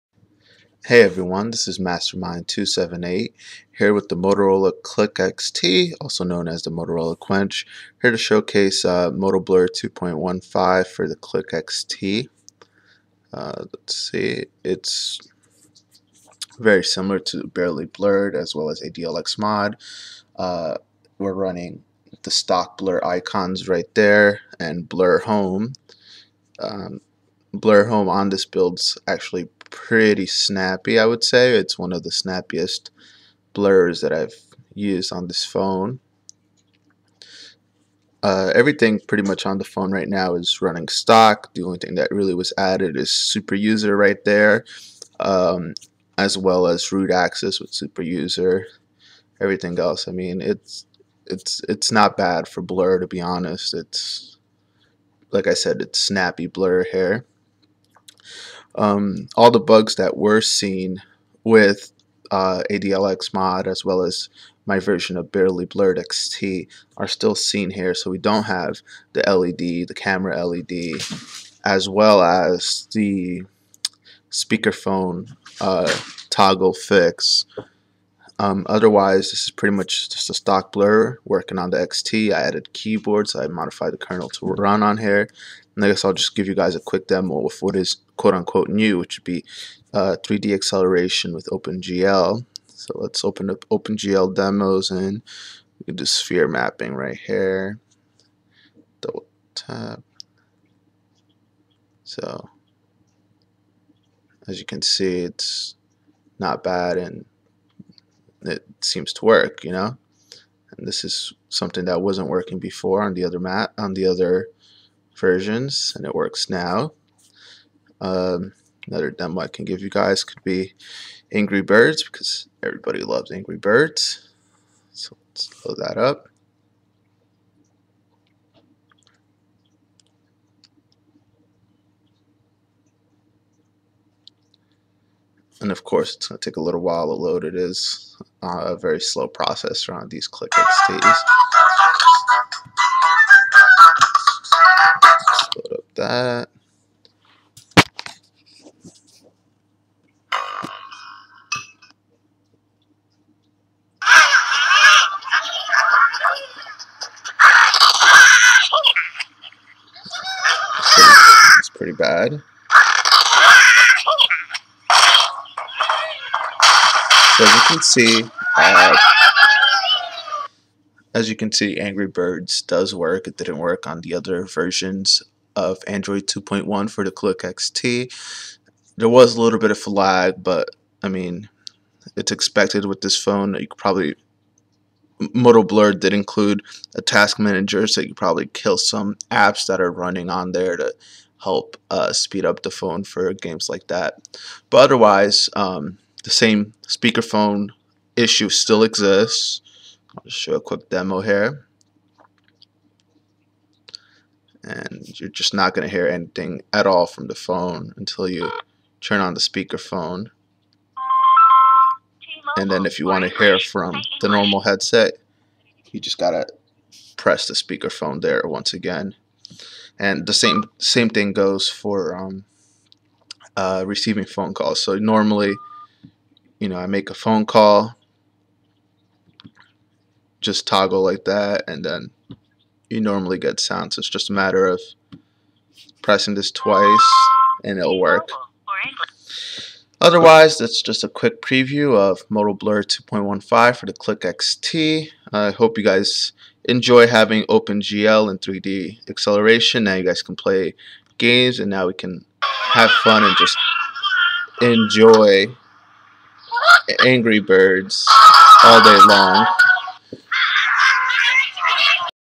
MotoBlur 2.1.5 for the CliqXT/Quench - Beta 1 ANDROID 2.1
**Beta 1**
1. So this ROM is a modification of the *official* Motoblur 2.1.5 released for the Motorola Cliq.
2. Added Root Access, everything else is STOCK.
3. Modified Kernel to run on the XT.
3. Keyboard Fix applied
4. Volume Rocker Fix Applied

So here is the low down on this rom:

WORKING:
3D Acceleration - OPENGL
3G Data
WIFI
Bluetooth
Camera/Camcorder
GPS
Accelerometer
Trackpad (Extremes only)
Proximity Sensor
Vibration

NOT WORKING
Camera LED (Sorry no FLASH on the Cliq)
Notification LED
Compass

Issues
Must toggle Speakerphone button in order to hear anything from the phone.
There is a temporary fix for the Speakerphone issue on the CliqXT by @firesnatch:
1. So you can answer a phone call and it will go to the normal speaker.
2. When you make a call, it will automatically go to Speaker and quickly go to normal speaker. If it doesn't just pull the phone away from your face and then bring it back and it will toggle to normal speaker.

3. Place ROM ZIP into the root of the SDCARD.
4. Boot into Recovery (Power + Camera Button)
5. Nand Backup your stock ROM.
6. Wipe *all* data (Including Data/Cache/Dalvick)
7. Install ZIP, pick this ROM.
8. Reboot System
9. Enjoy.

Want to Improve Battery Life?
1. Boot into Recovery (Power + Camera).
2. Wipe Battery Stats.
3. Let the battery fully discharge.
4. Let it charge Completely! Then use normally.